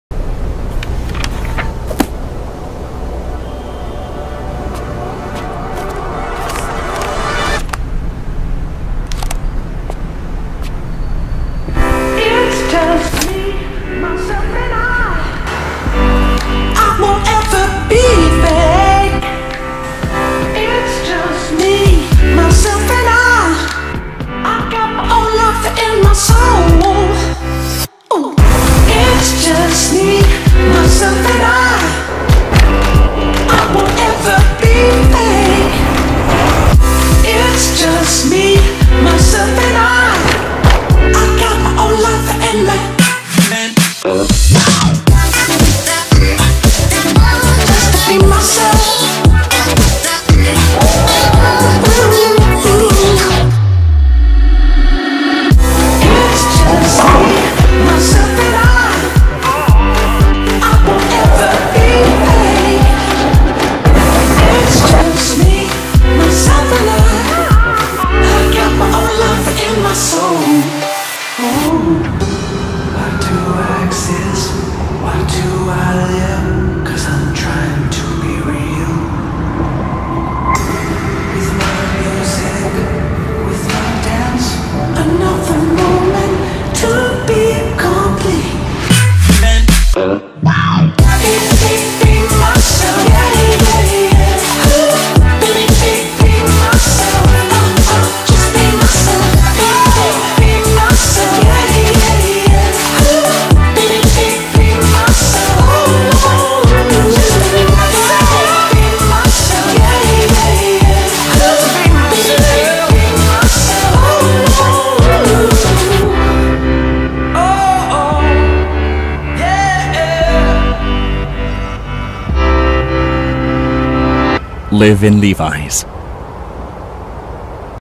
[Morph] Sung Ye Jin 성예진 | 2018 Levi's X KAI Campaign Film
이번 Levi's X KAI 캠페인에 몰프모델 성예진이 함께 했습니다. 영상 속 발랄한 그녀!